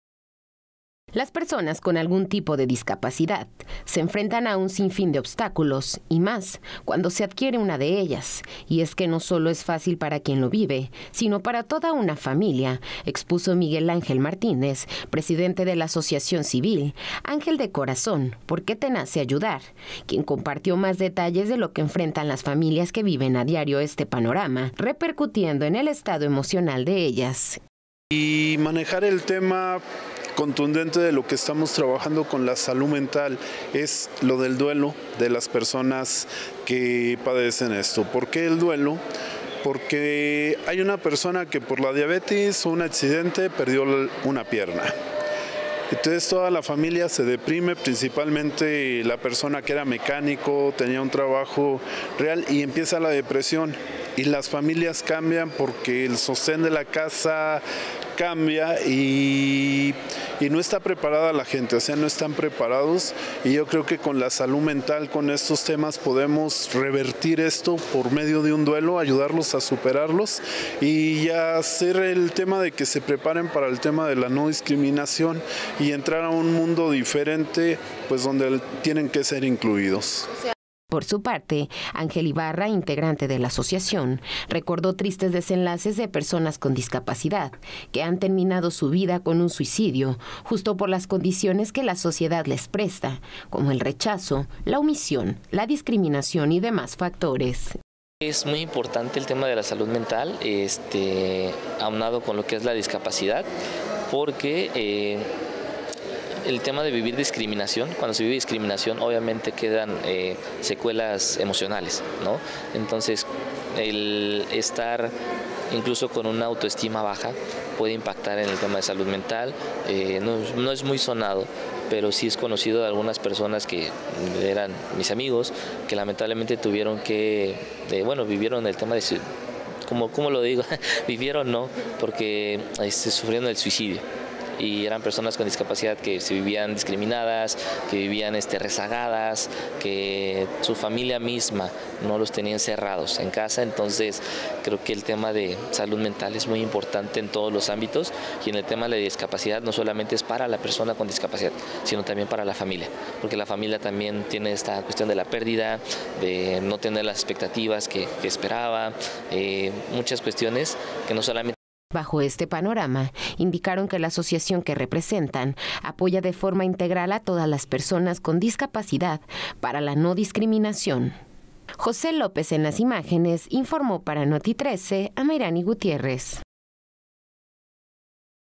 Discapacidad de nacimiento o adquirida, cómo se afronta.
Salud 🔵 Cómo afrontan el duelo las familias que tienen una persona con discapacidad, más cuando con esta no se nace y se adquiere.